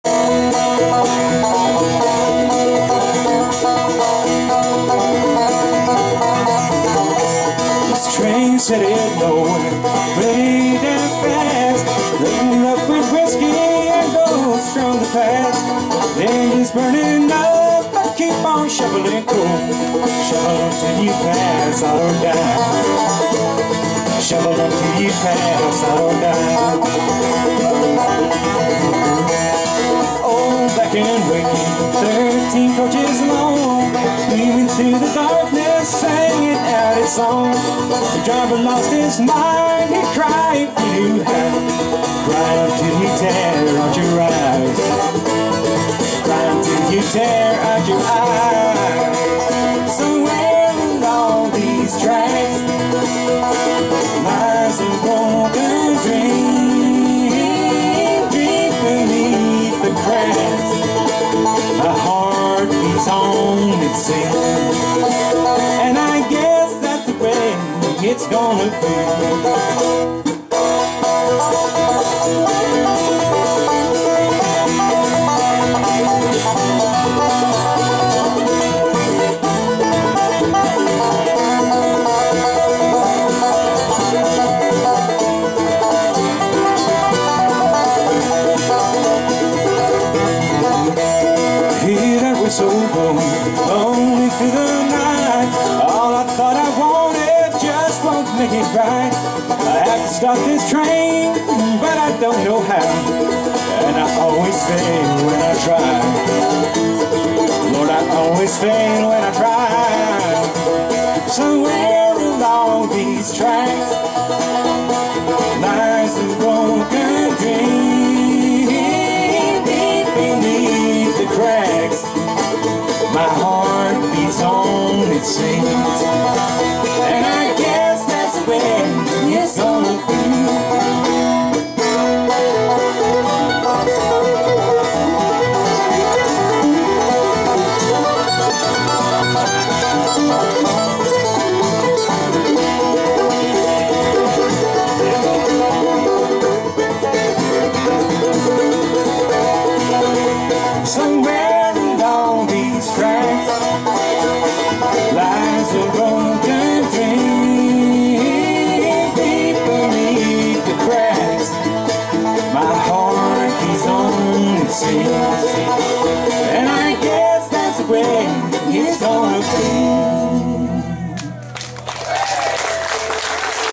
Moonshine Brothers performing at Grenna Bluegrass festival 2013
Gränna, Sweden, 17/08 2013. See more videos from Grenna Bluegrass festival 2013 on my channel.